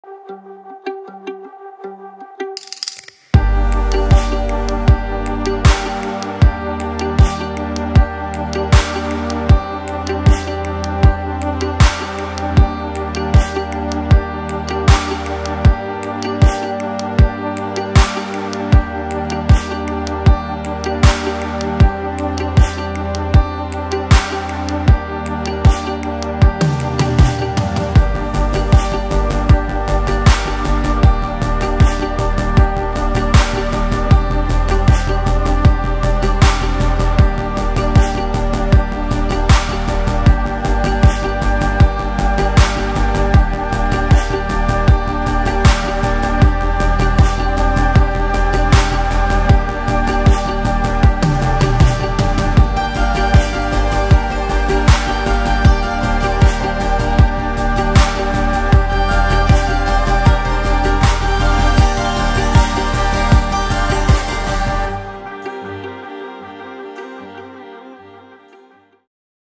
Electric machines - Komatsu at bauma 2022
Responding to the rising concern for global climate change, Komatsu has taken the initiative to invest all the knowledge and experience we acquired over the past century into developing solutions that can help move the construction industry towards carbon neutrality. The Komatsu electrified machines that will be premiered at bauma 2022 will be another testimony of the company’s engagement towards reducing harmful emissions worldwide.